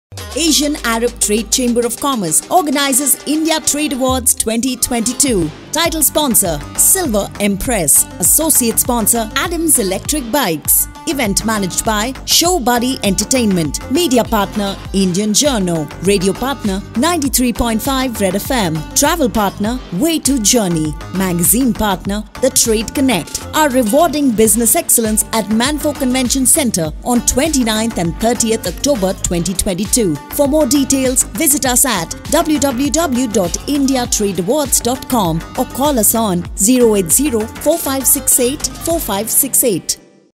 It's our heartiest pleasure to announce our partners for the India Trade Awards 2022.
Date- 29th & 30th, October
We are happy to have the support from all partners -
Event Management Partner - Show Buddy Entertainment Pvt. Ltd.
Media Partner - India Journo
Radio Partner - Superhits Red FM 93.5
Travel Partner - Way 2 Journey India Pvt. Ltd.
Magazine Partner - The Trade Connect